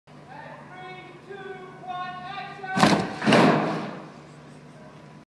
Breakaway door
Breakaway door designed to buckle when kicked